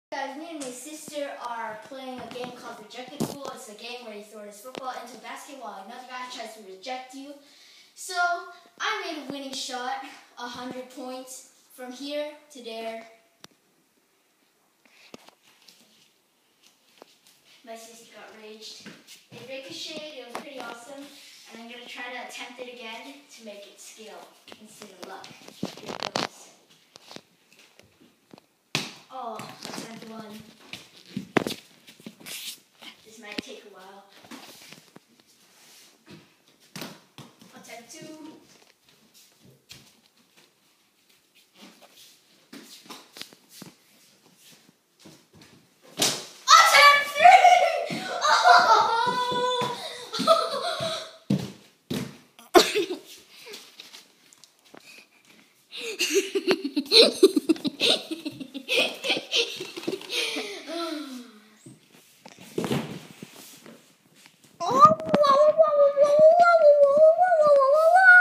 Epic Shot
In the game that I made up with my sister, I made the most amazing shot